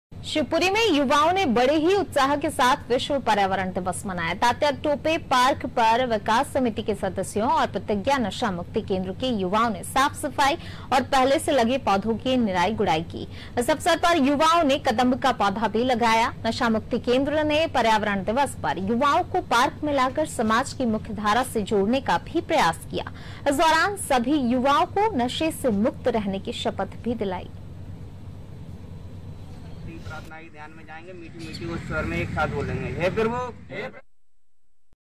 शिवपुरी : विश्व पर्यावरण दिवस का उत्साह | Top News today Hindi | Breaking News Hindi | Mp News Hindi
शिवपुरी में युवाओं ने बड़े ही उत्साह के साथ विश्व पर्यावरण दिवस मनाया... तात्या टोपे पार्क पर विकास समिति के सदस्यों और प्रतिज्ञा नशा मुक्ति केंद्र के युवाओं ने साफ सफाई...और पहले से लगे पौधों की निराई-गुड़ाई की...इस अवसर पर युवाओं ने कदंब का पौधा भी लगाया...नशा मुक्ति केंद्र ने पर्यावरण दिवस पर युवाओं को पार्क में लाकर समाज की मुख्य धारा से जोड़ने का भी प्रयास किया...इस दौरान सभी युवाओं को नशा से मुक्त रहने की शपथ भी दिलाई...।